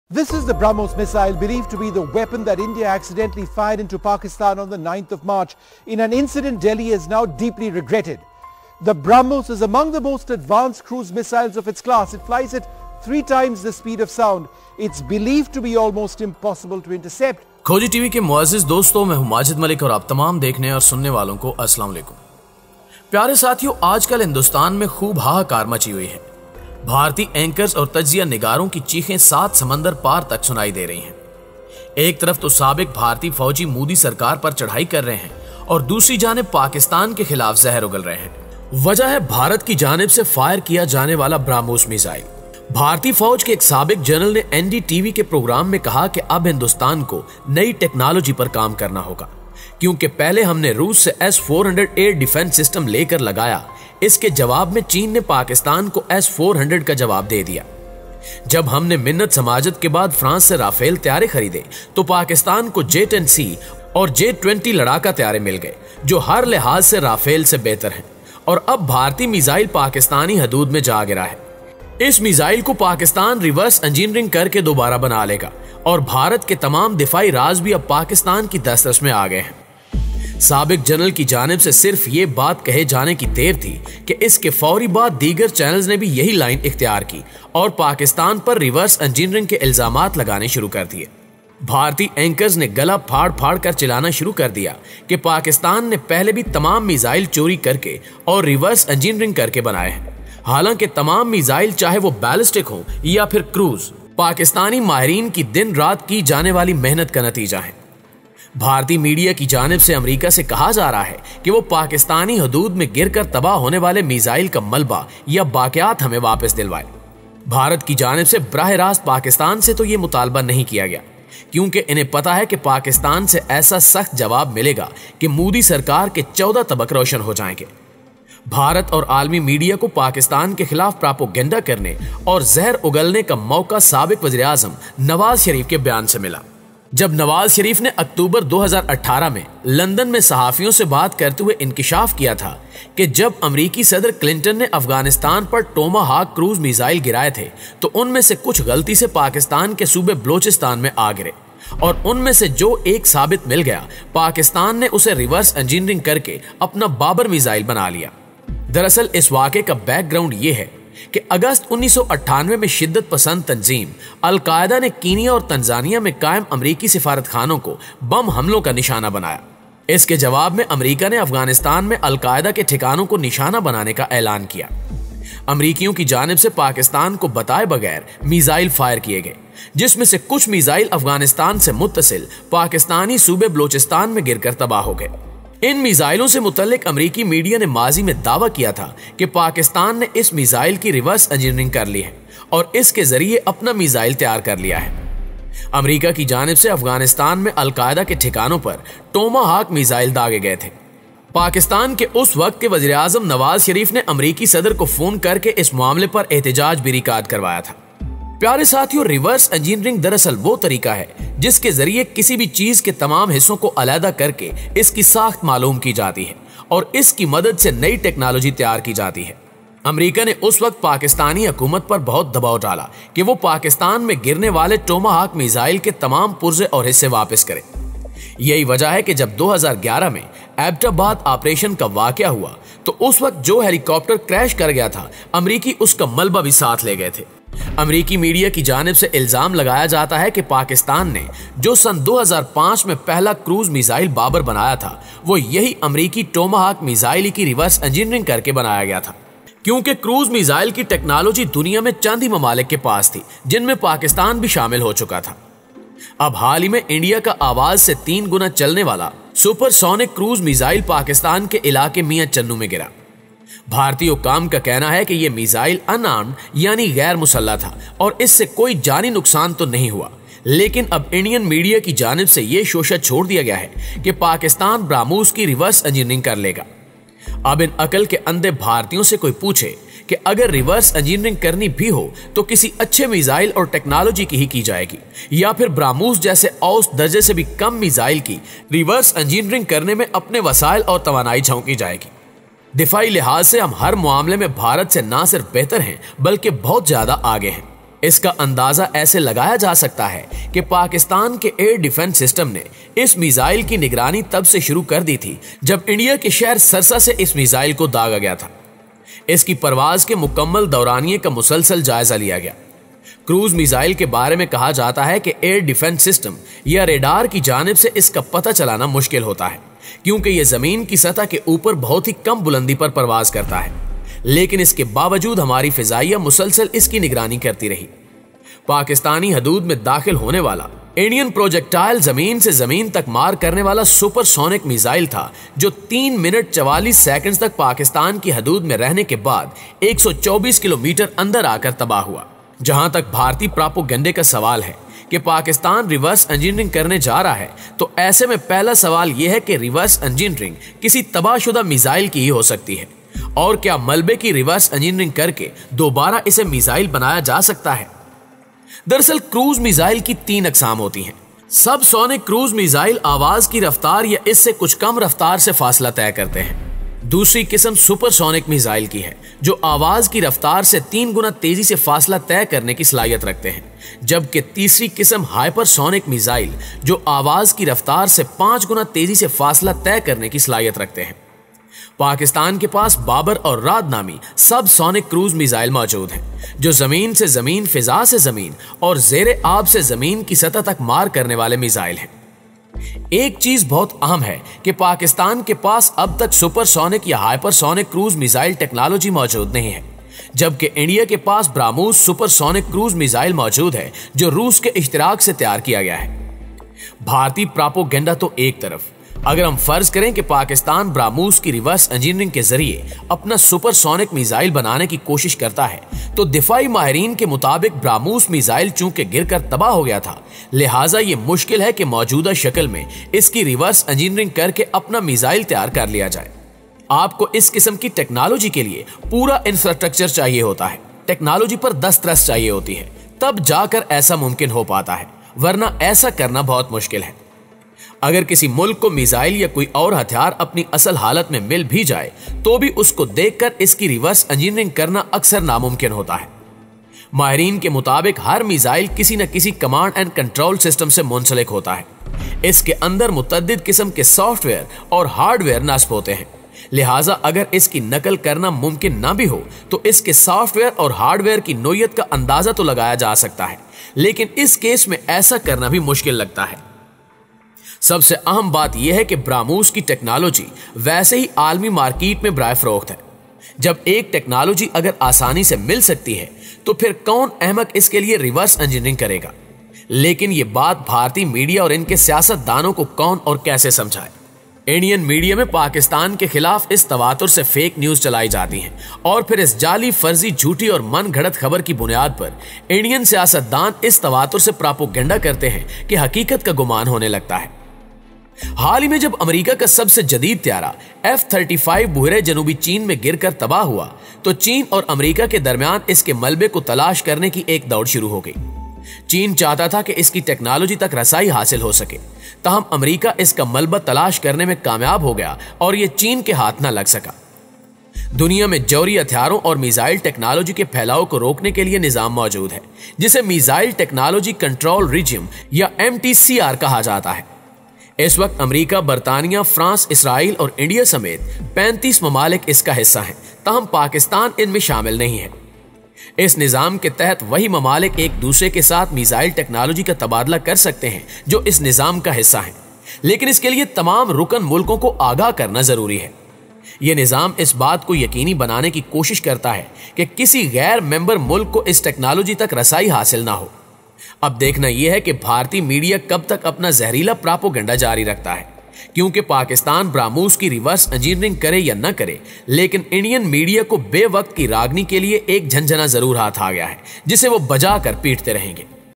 Can Pakistan & china Copy Brahmos Missile? Reverse Engineering? KHOJI TV
Can Pakistan & china Copy Brahmos Missile
Reverse Engineering of Brahmos
Khoji TV is a project of Talha Films
Our Team